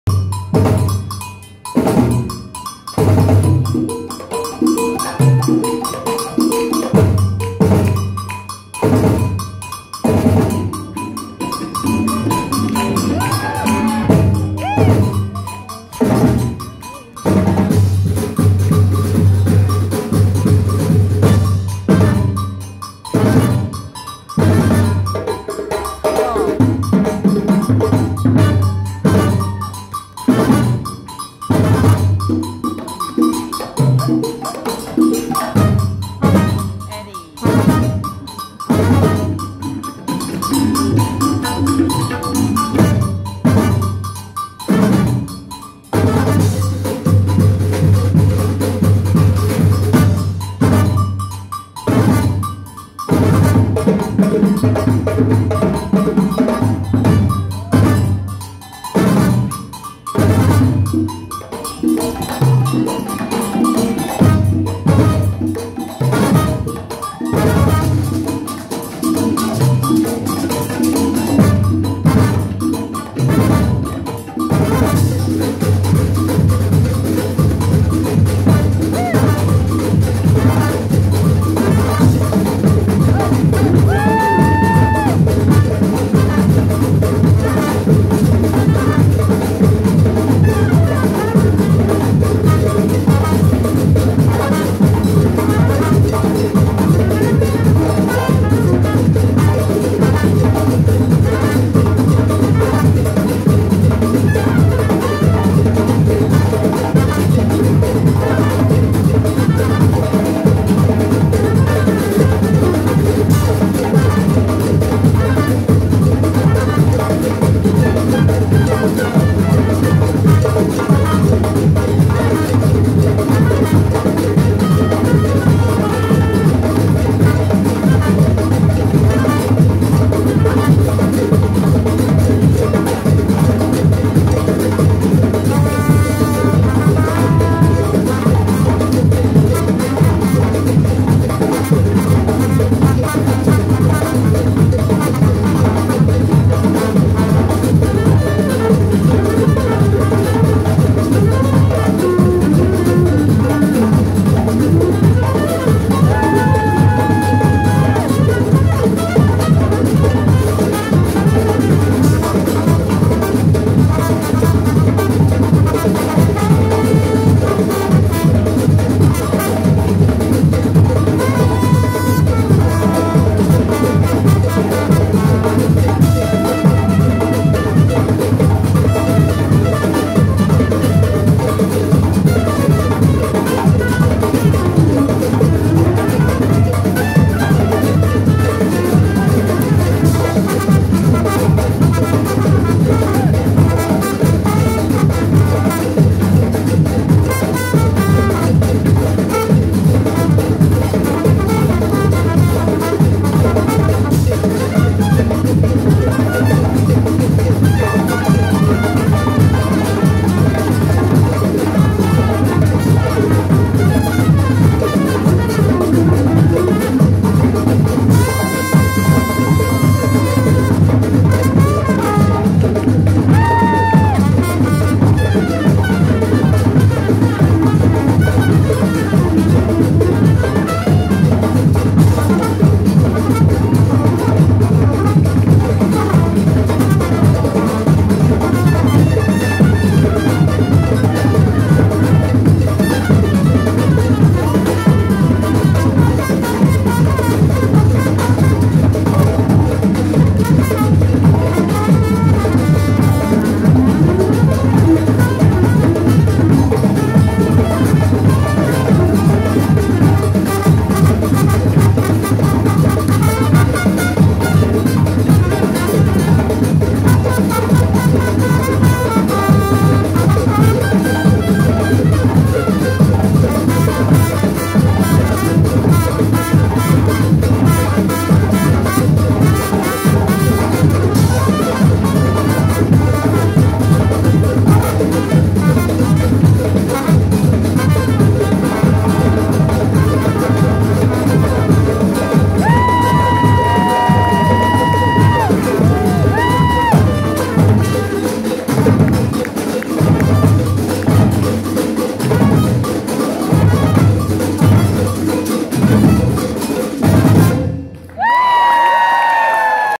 Spoken Hand Percussion Orchestra with Hip Hop Dancers
With hip hop dancers from Kùlú Mèlé: Eddie Smallwood, Bryant Lee, and Yusuf Young
Bata: John Wilkie, Ken Fauntleroy, Ishmael Jackson
Djembe: Steve Jackson, Omar Harrison, Daryl Burgee
Samba: Tom Lowery, Baba Joe Bryant, Ron Howerton, Alex Shaw
Tabla: Dan Scholnick, Debanik Purkayastha, Lenny Seidman
Trumpet:Josh Lawrence
Trombone: Brent White
Guests from Rio: Guilherme and Gustavo Oliveira